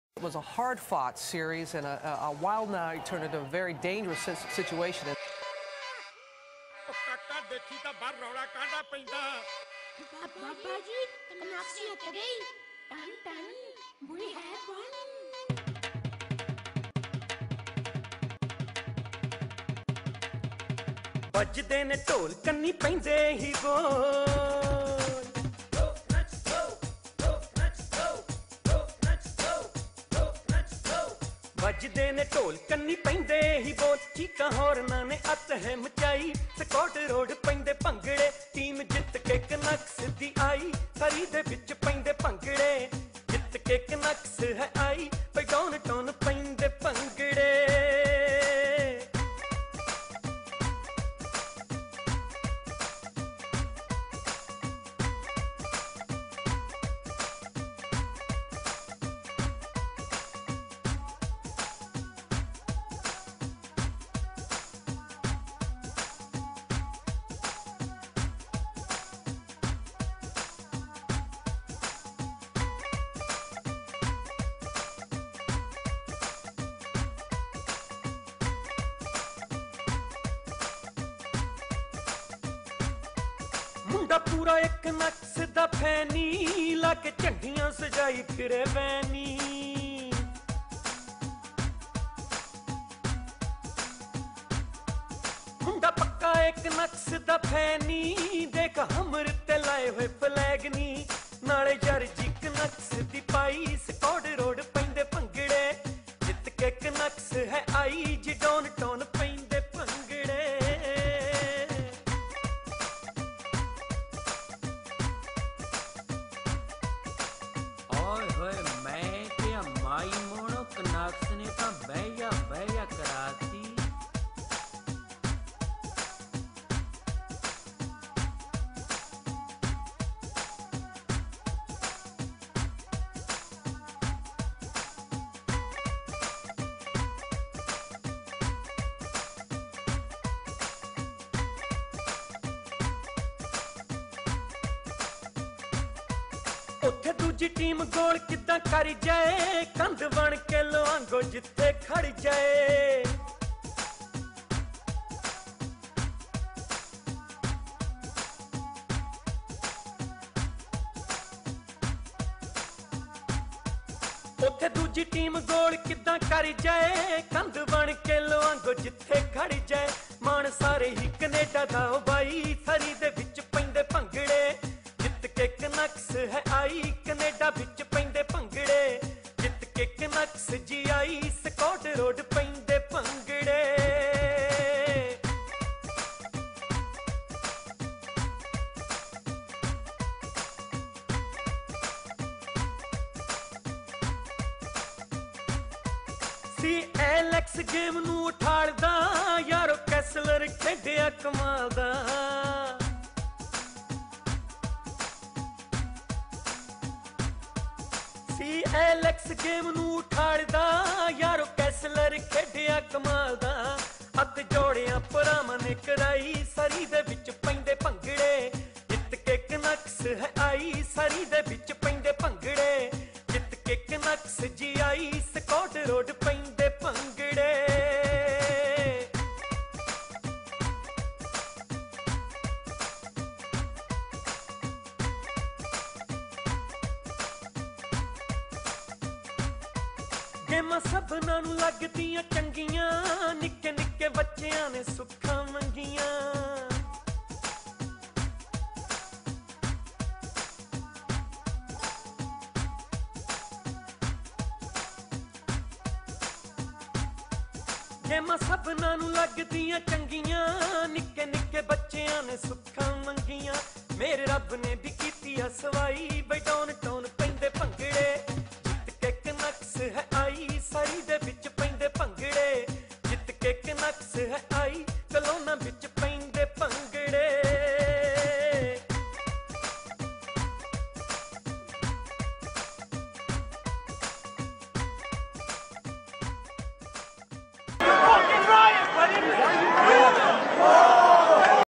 Funny Vancouver Riot Music Video
Summary of the Vancouver Riots... with a song!